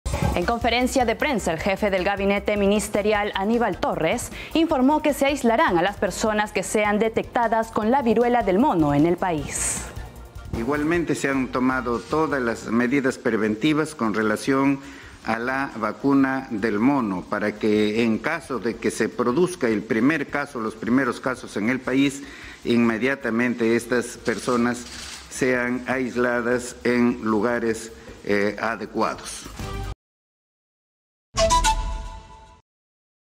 🔴🔵Viruela del mono: Gobierno dispone aislamiento para eventuales primeros casos de la enfermedad
El presidente del Consejo de Ministros, Aníbal Torres, indicó que el Gobierno viene implementando diversas medidas ante la eventual detección de los primeros casos de la viruela del mono en el Perú. En esa línea, señaló que las personas que tengan dicha enfermedad deberán cumplir con un periodo de aislamiento para evitar el incremento de casos.

Exitosa Perú
"La voz de los que no tienen voz"

Corporación Universal